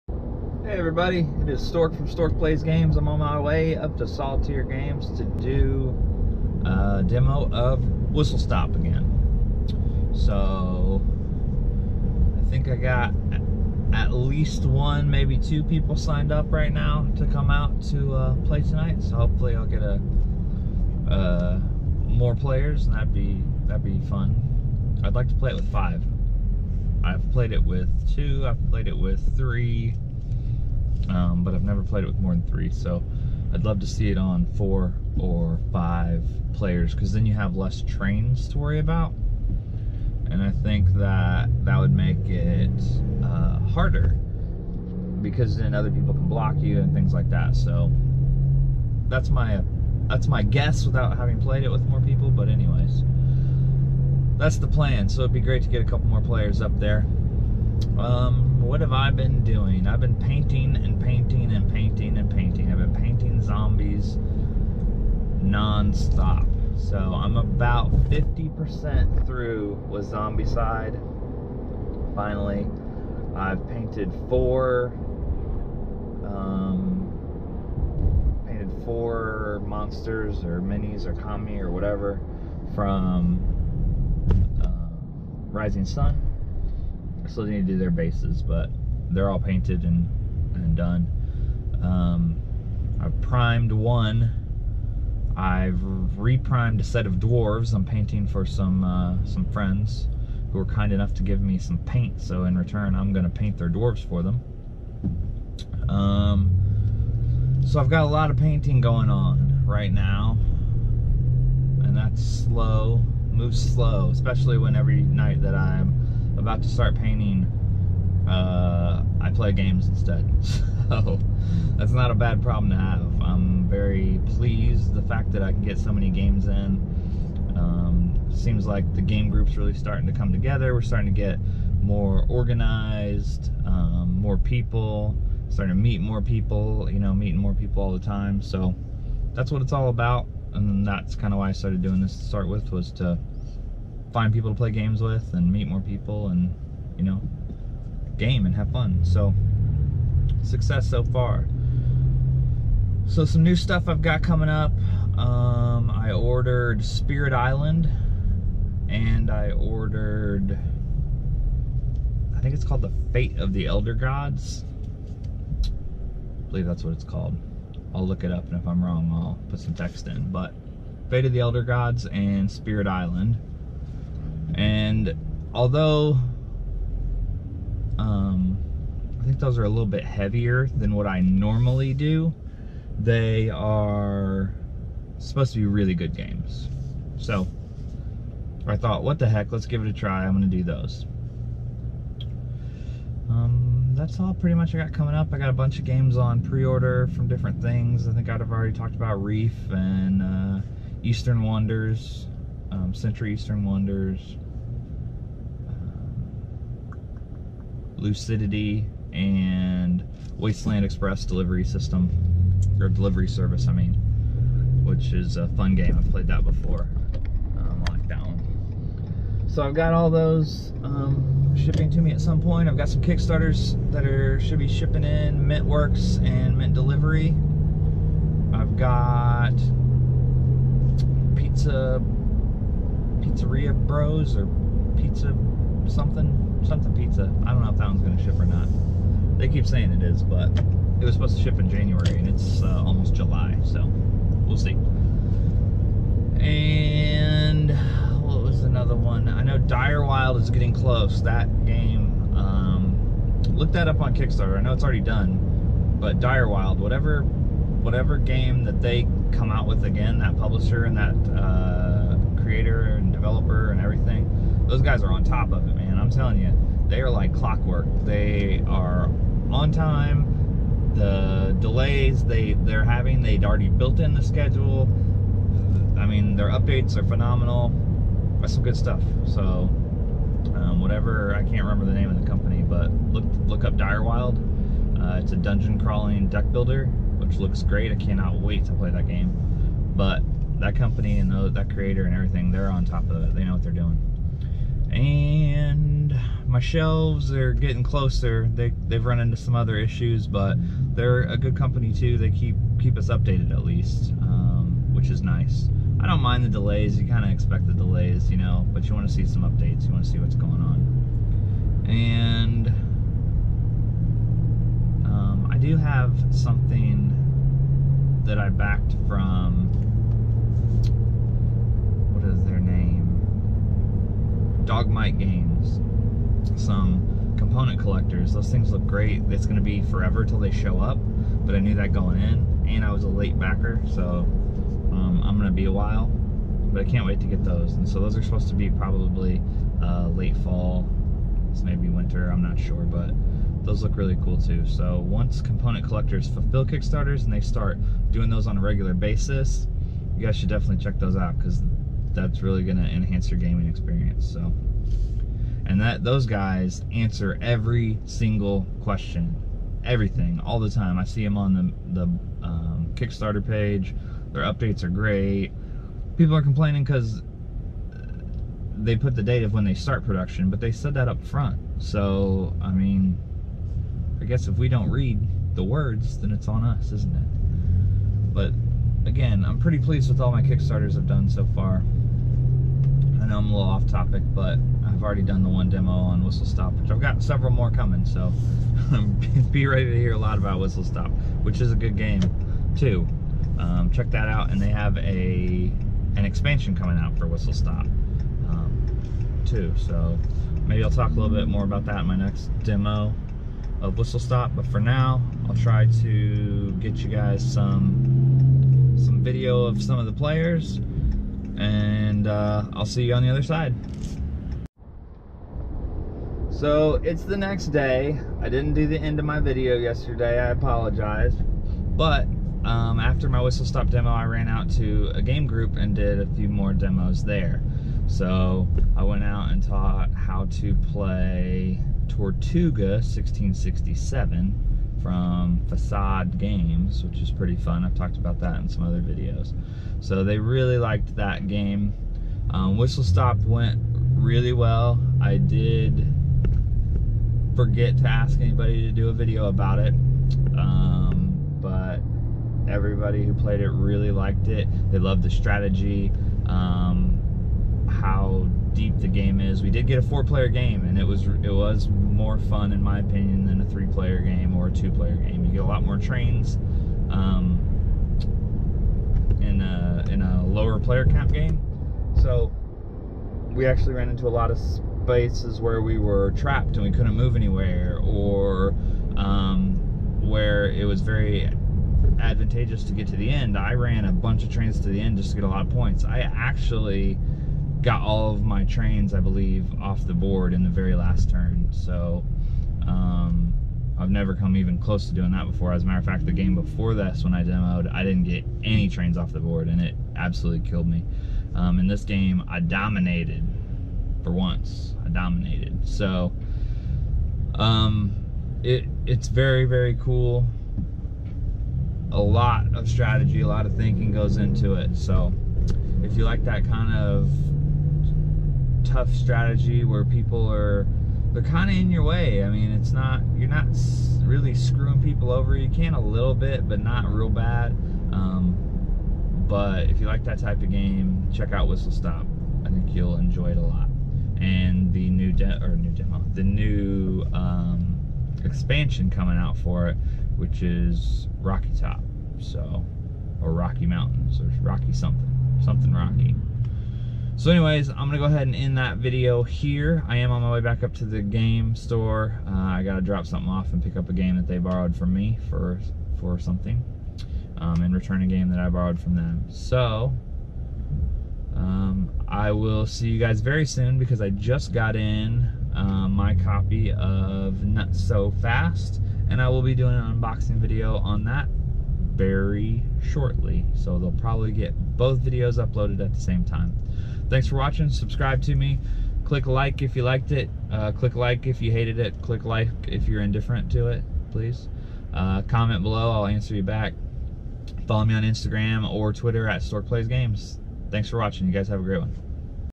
Vlog 6/27/18 Whistle Stop demo
I demo Whistle Stop again and talk about kickstarters and other such things.